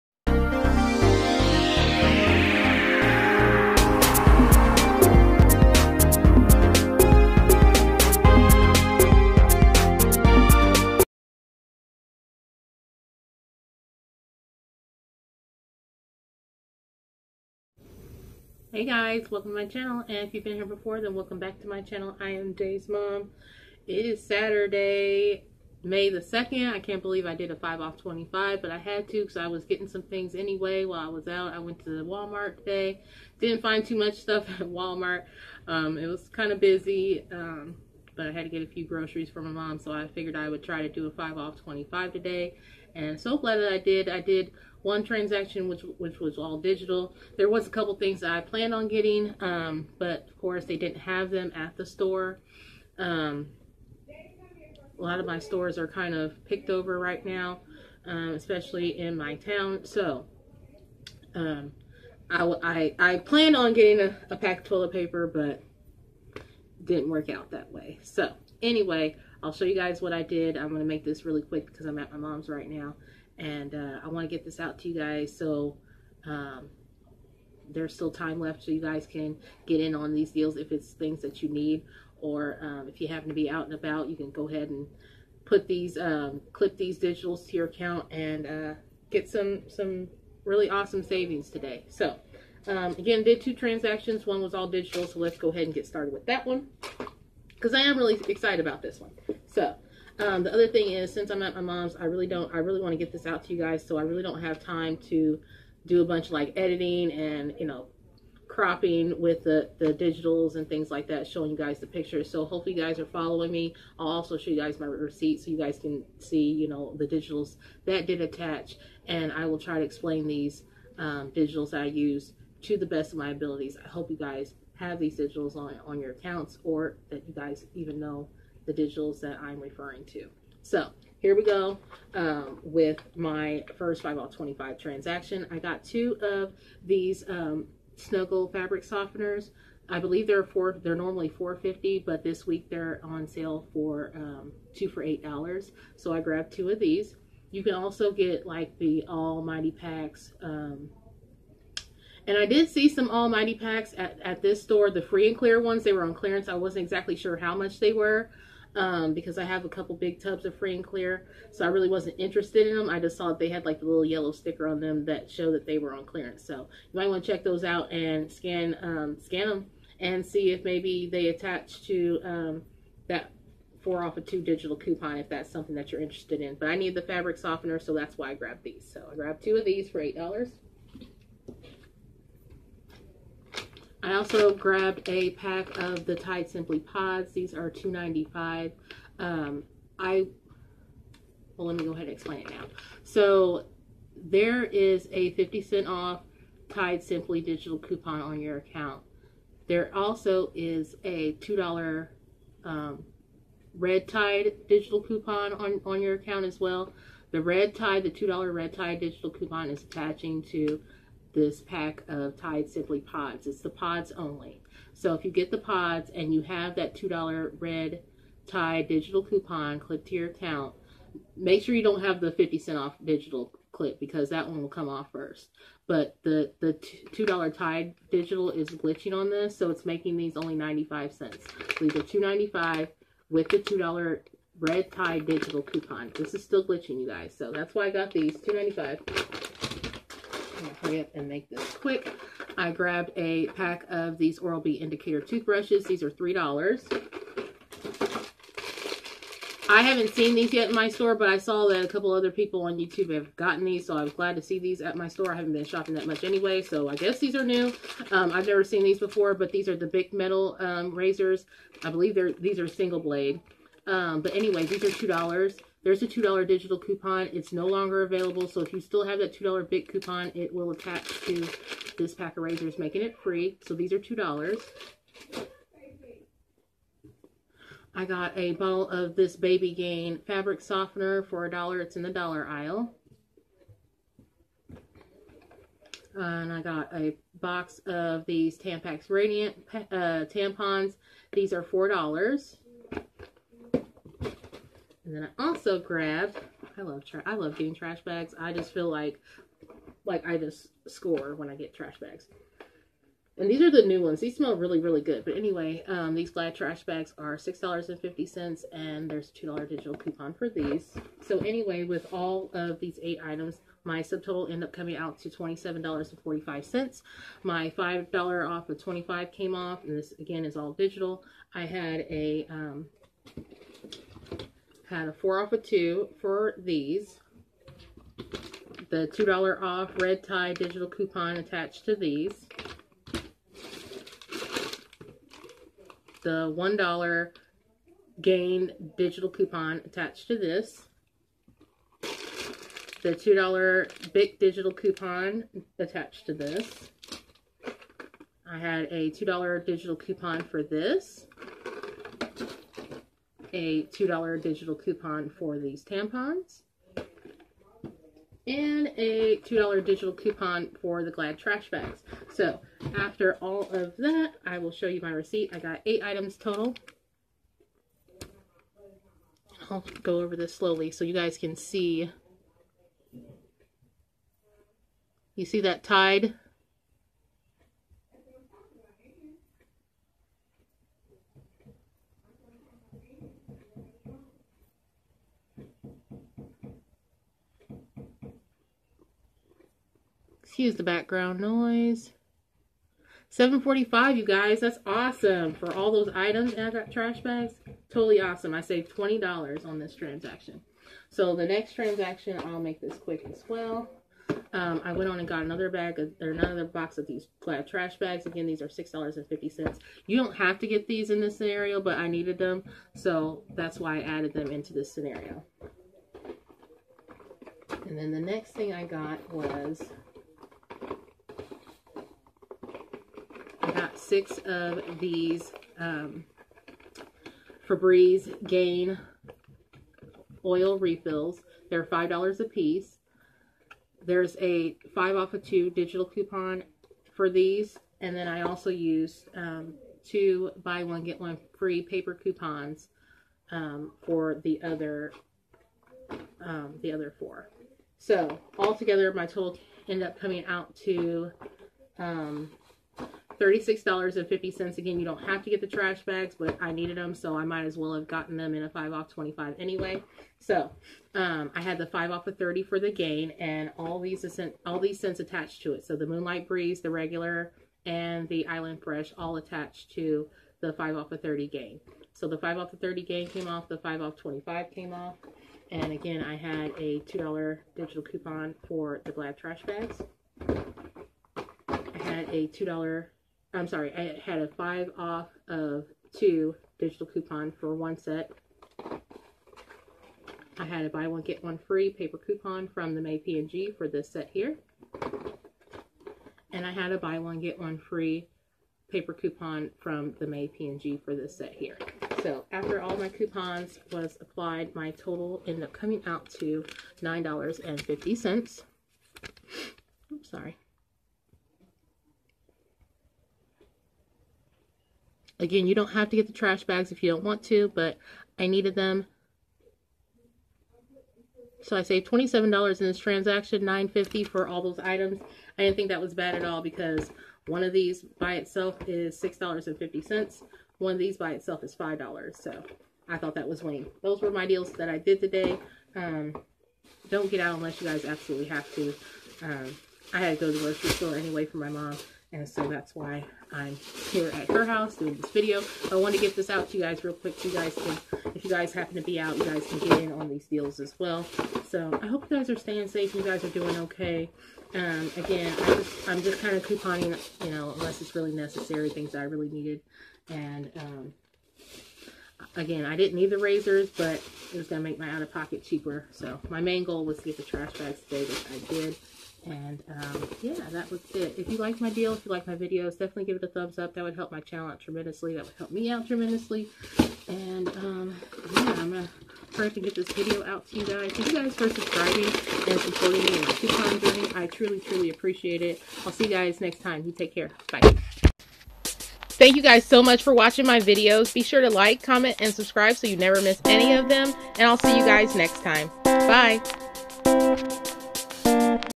MY DOLLAR GENERAL ESSENTIALS $5 OFF $25 DEALS 5/2
Hey everyone 👋👋 here's my DG $5 off $25 deals I did today. Hope everyone is staying safe. DON'T GO OUT UNLESS NECESSARY!! One deal all digital. Enjoy the vid, and as always...

Xoxo 💋

Let's win together with Fetch rewards. Get $2 back when you scan your first receipt on the app.